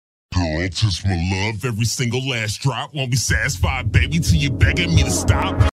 guess what im animating next
go ahead, guess.

the character is chrome beats btw shes a demi demon dog mom